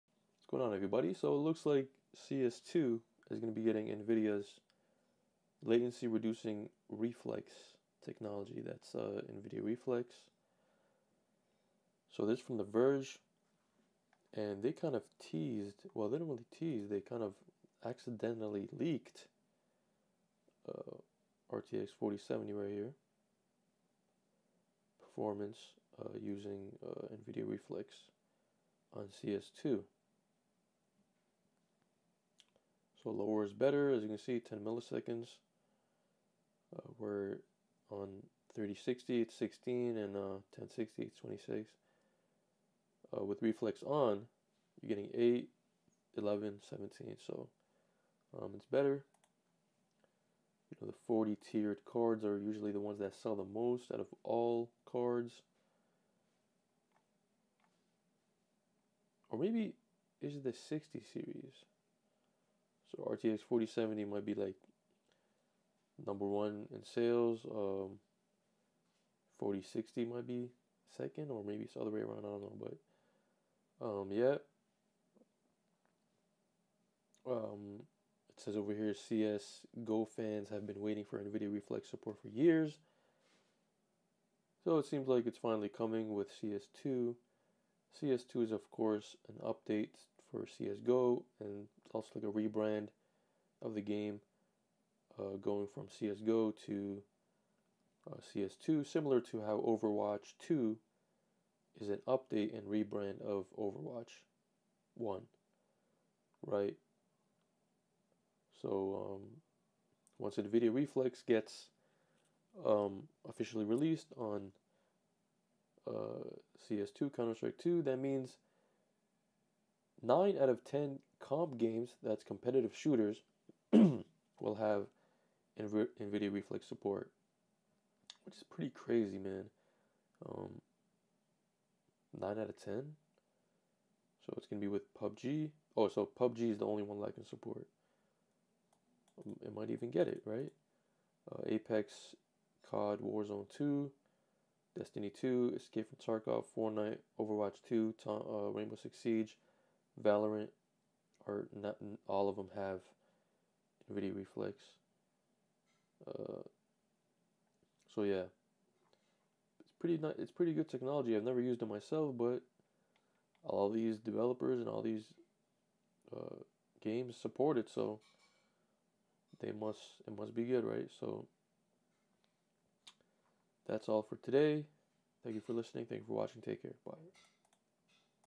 Counter-Strike 2 is getting Nvidia Reflex
Counter-Strike: Global Offensive never got Nvidia Reflex, but its replacement Counter-Strike 2 will get the important latency upgrade.